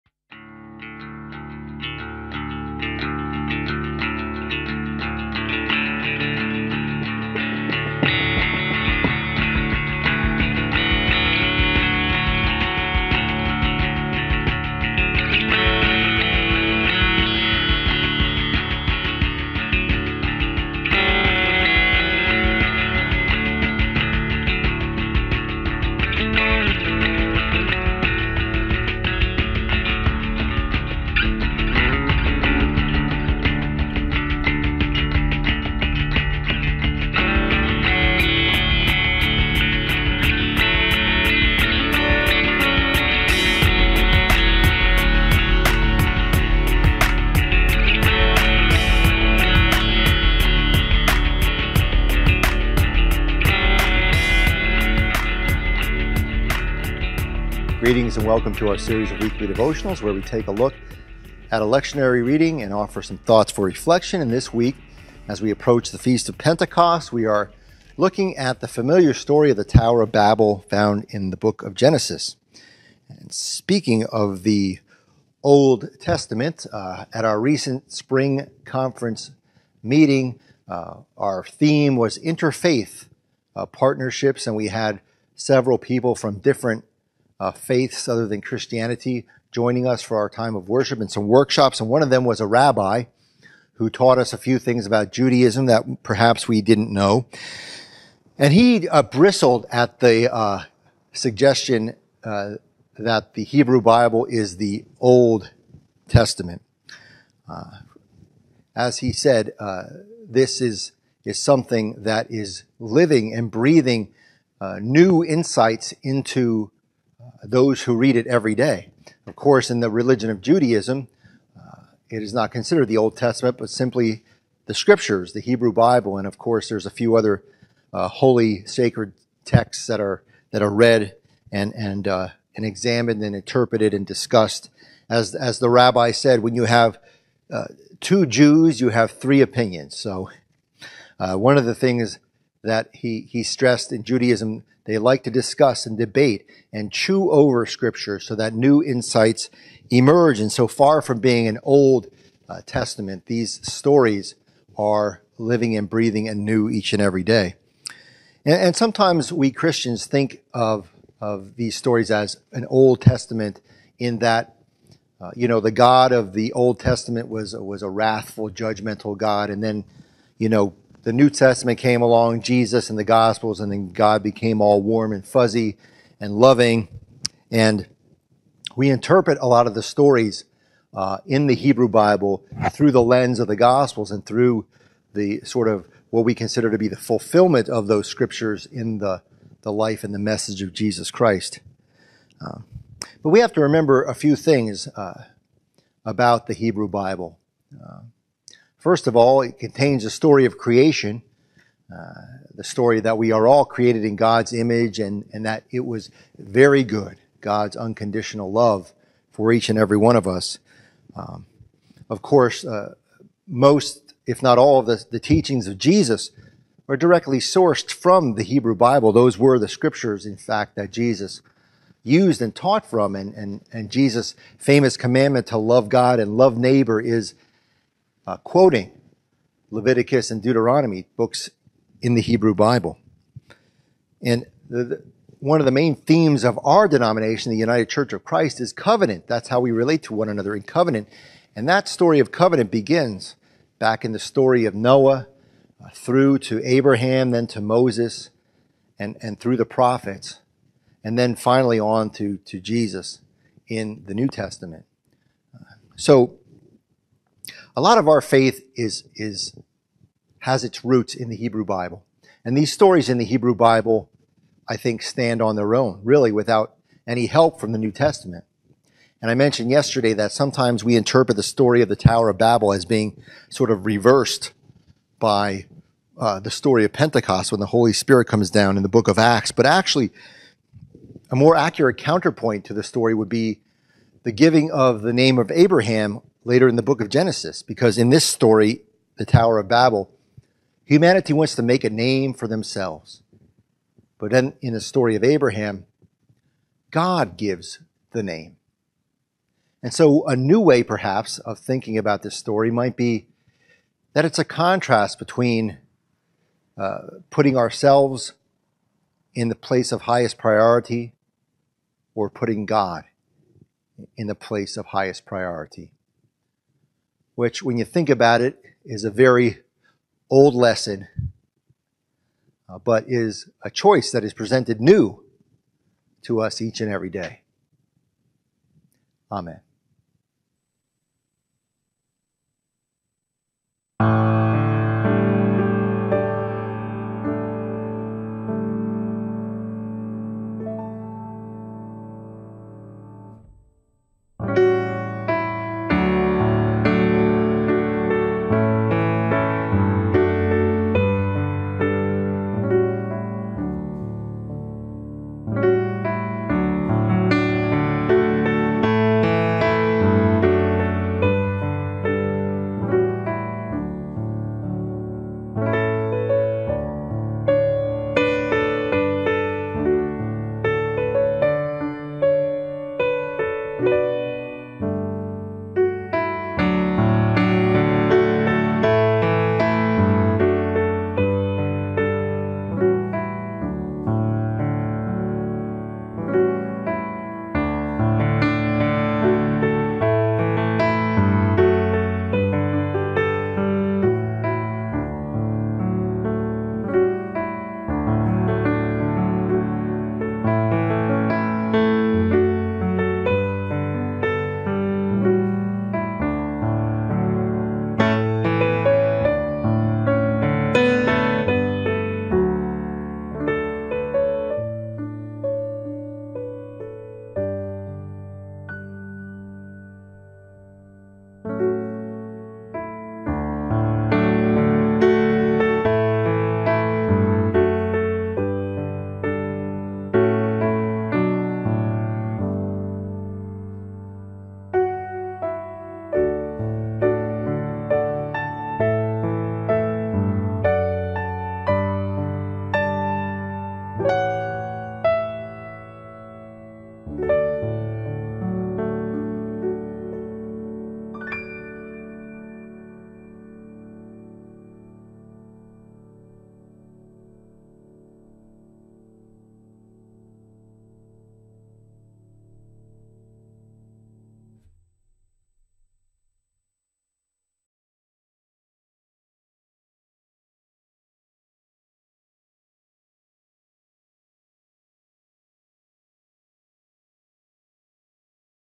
Forks United Church of Christ Daily Devotional - May 31, 2022
Forks UCC hopes to care for your heart, mind, and soul and give you strength with daily devotionals.  Presented by Reverend Robert Gutekunst, Music by Anthony Newett, Produced by Newett Studios,